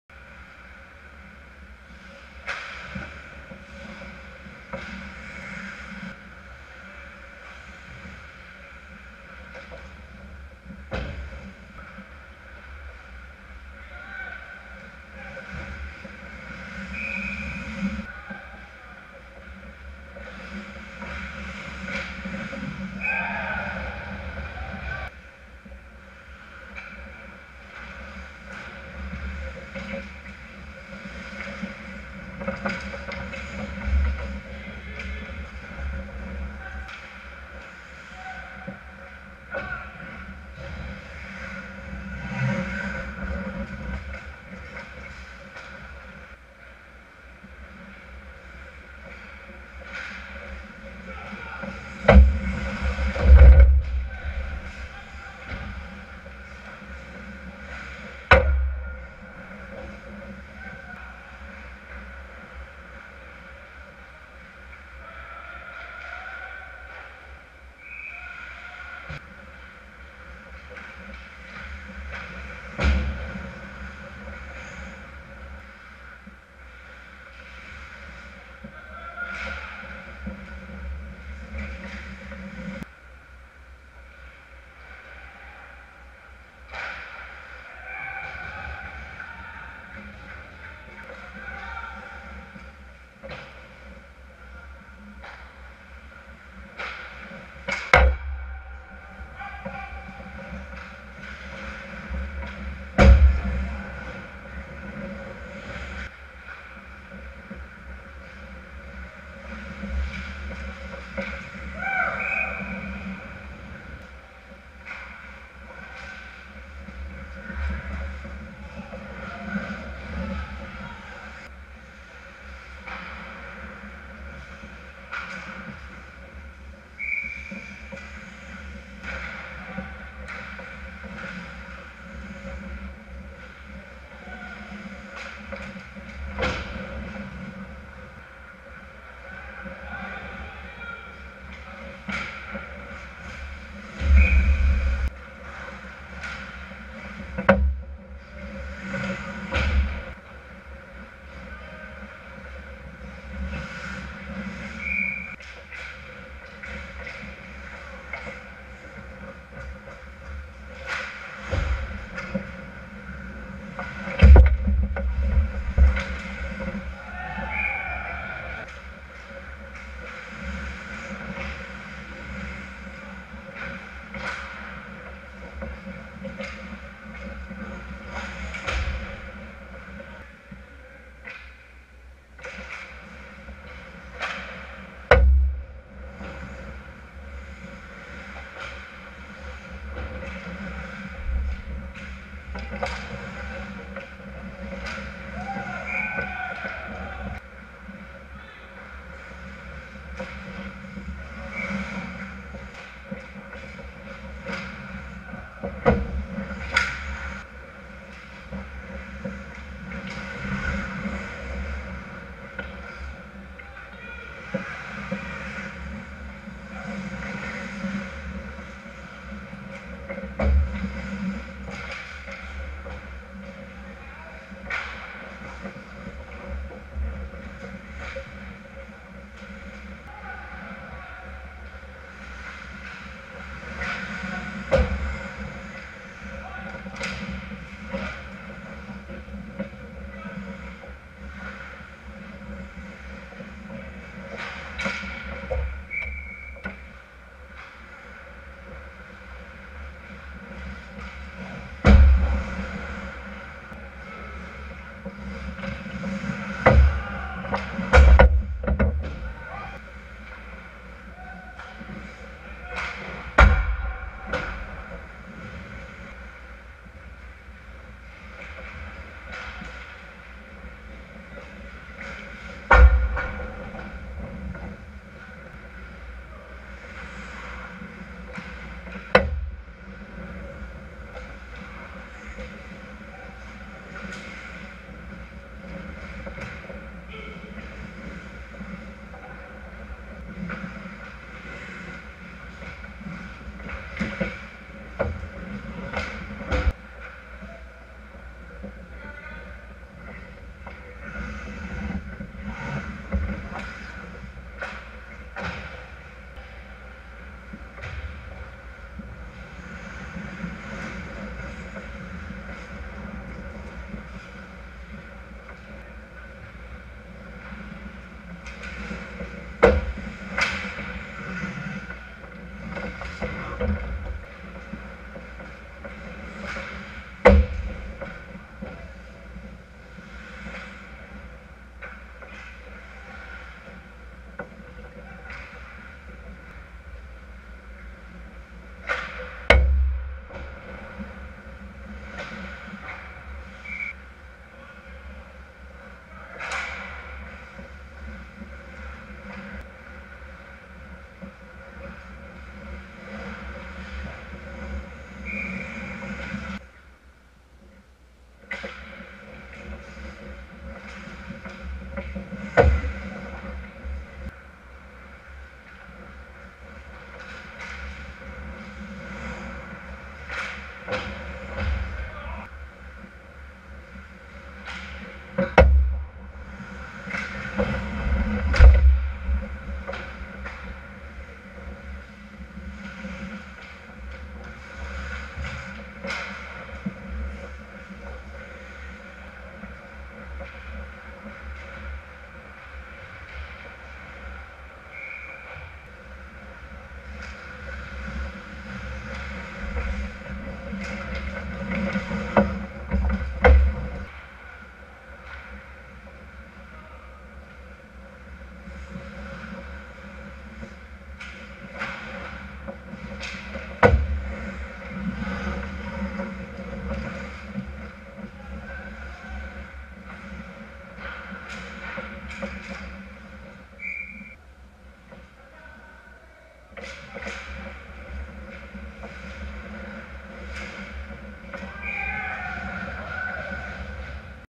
PWICE Upper B: Blazers vs Honey Badgers 9/23/2014
Blazers faced off against Honey Badgers and after a high-scoring first period left the teams 2-2, Blazers took the lead 4-2 in the second and then clamped down defensively in the third to close out the game. Ben scores twice and Jay and new sub Mike each get a goal. I stopped 24 of 26 shots.